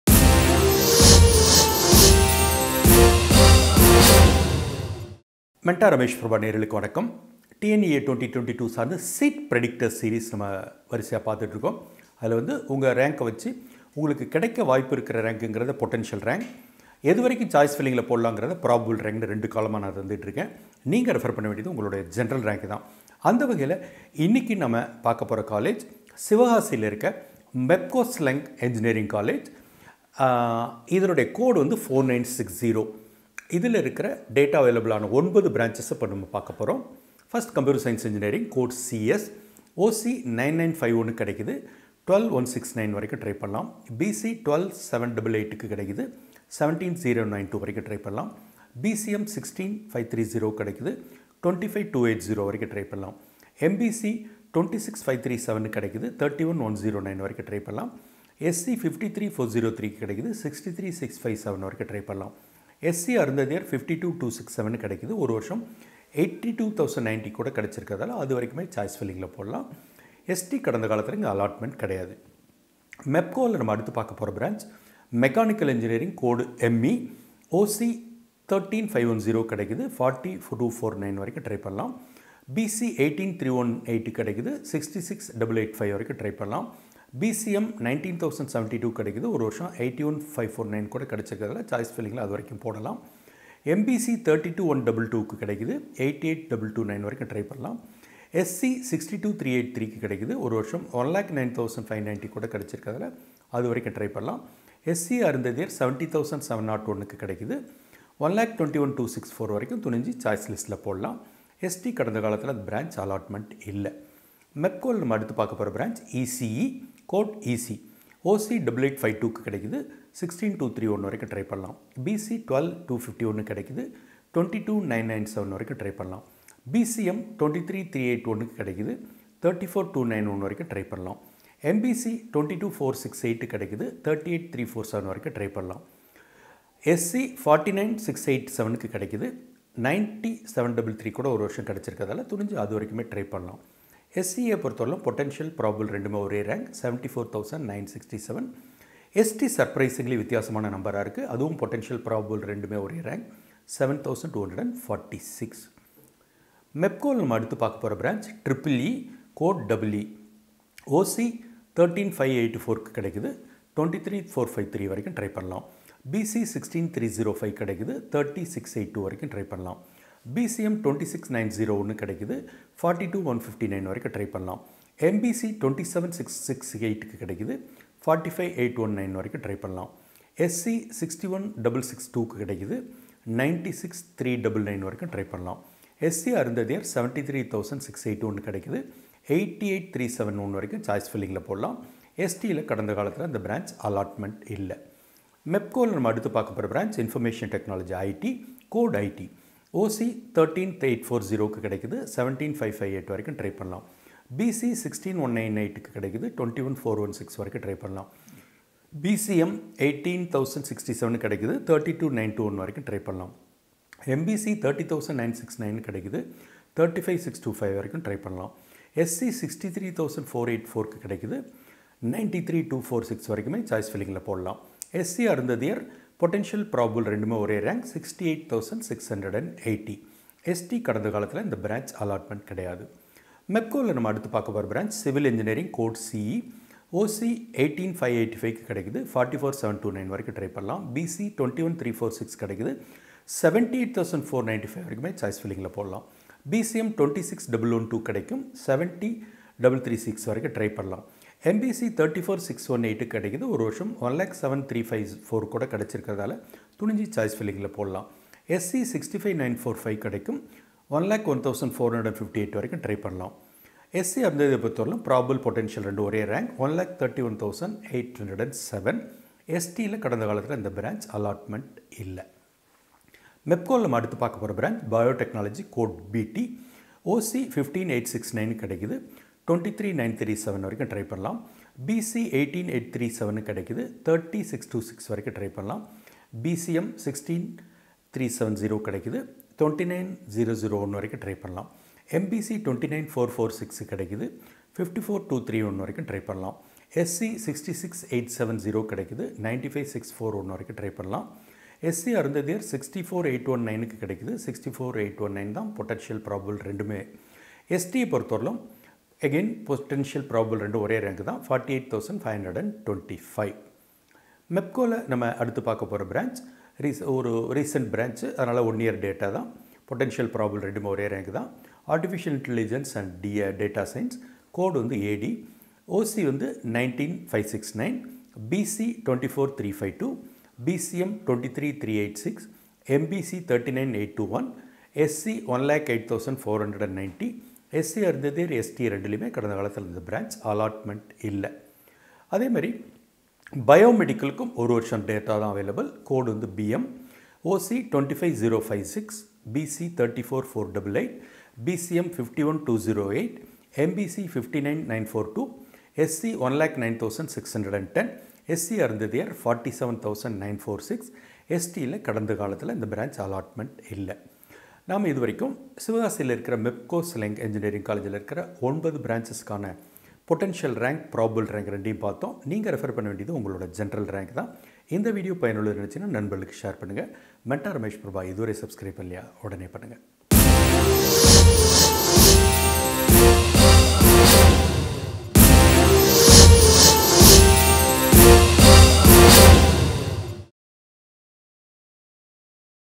Seat Predictor MEPCO Sivakasi | Check with your Rank | TNEA 2022 | Mentor Ramesh Prabha
Educationist Ramesh Prabha is giving Seat Predictor for Mepco Schlenk Engineering College (Autonomous), Sivakasi,  with potential and probable Ranks based on Ranks of 4 years(2017, 18, 19 & 2020) to help choice filling  in TNEA 2022.